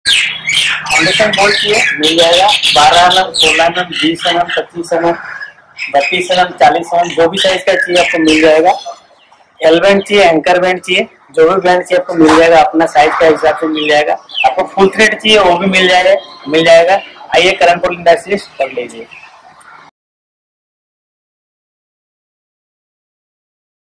Manufacturer of Foundation Bolts at The Karan Pole Industries, Tinsukia
We at The Karan Pole Industries manufacture Foundation Bolts of 12mm, 16mm, 20mm, 25mm, 30mm, 32mm and 40mm.
All lengths of threading are possible, half-length, full length or custom-length.
Any type of bend, be it L Bend, U Bend, Closed Eye Bend, Open Eye Bend, Square Bend or any Custom Bend is possible.